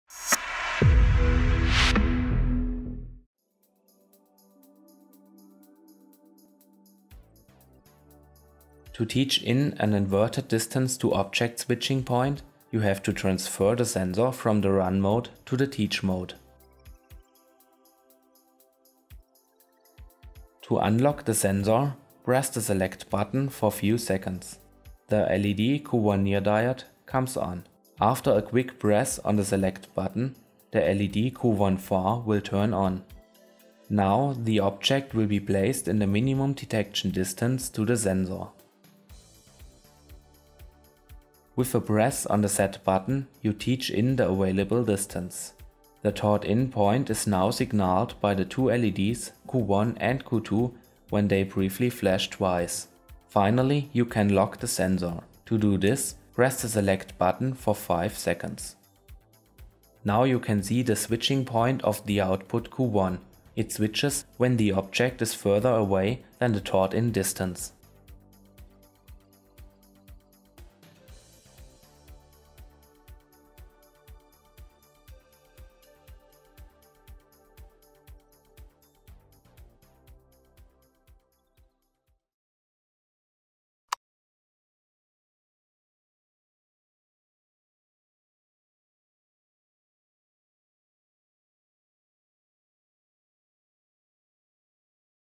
Tutorial Dx35 (Part 4/7): Inverted one-point (DtO) teach | SICK AG
This video describes how to perform an inverted one-point (Distance to Object = DtO) teach for SICK’s Dx35 distance sensor product family.